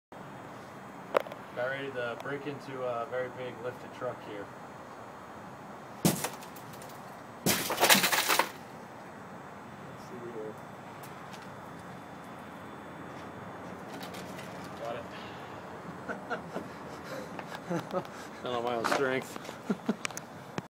Breaking Window
Soy cousin locked the keys in the truck and we tried to slim Jim it at that didn't work so I tried to slide it through the window area to unlock it and popped the window. Oh well. Lots of fun to break.